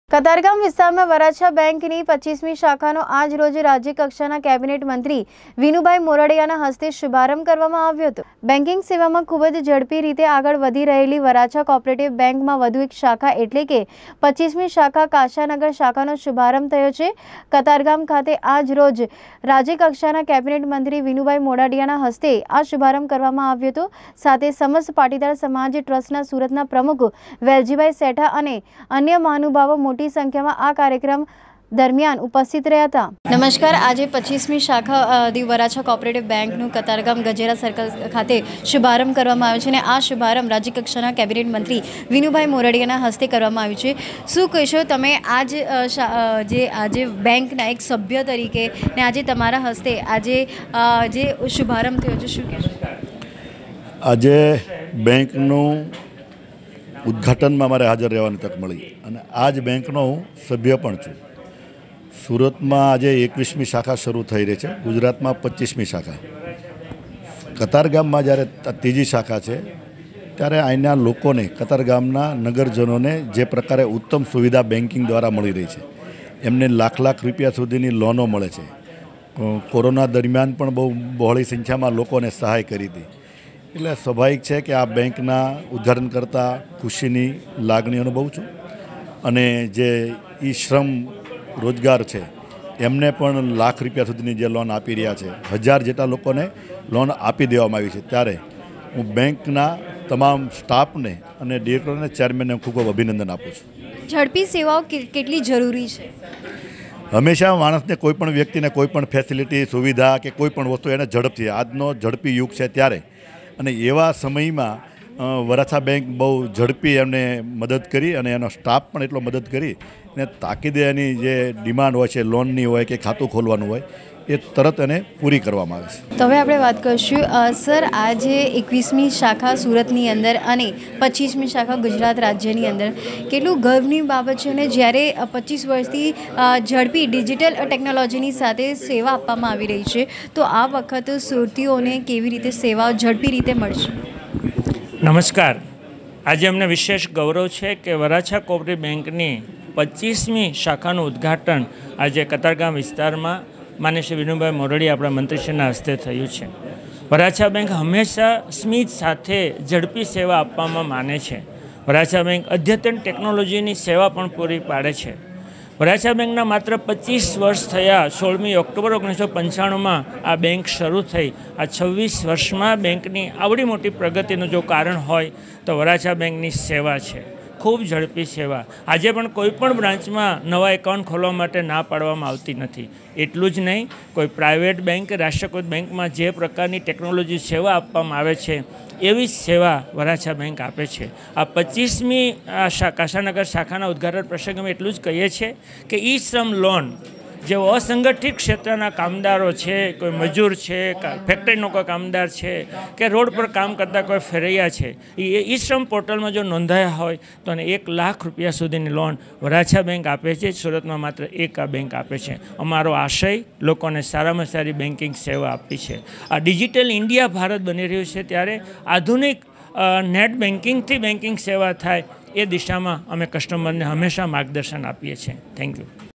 AB NEWS - કતારગામ વિસ્તારમાં વરાછા બેંકની 25મી શાખાનો આજરોજ શુભારંભ કરવામાં આવ્યો.
કતારગામ વિસ્તારમાં વરાછા બેંકની 25મી શાખાનો આજરોજ શુભારંભ કરવામાં આવ્યો.રાજ્યકક્ષાના કેબિનેટ મંત્રી વિનુભાઈ મોરડીયા ના હસ્તે શુભારંભ કરવામાં આવ્યો.